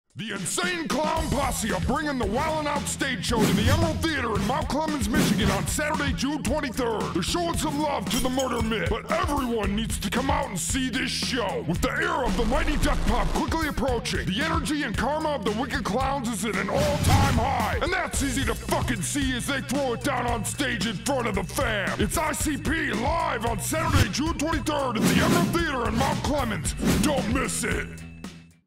SATURDAY, JUNE 23rd! ICP LIVE IN CONCERT AT THE EMERALD BALLROOM IN MT. CLEMENS
The Insane Clown Posse are bringin' their wilin' out stage show to the Emerald Theatre in Mt. Clemen's, Michigan on Saturday, June 23rd!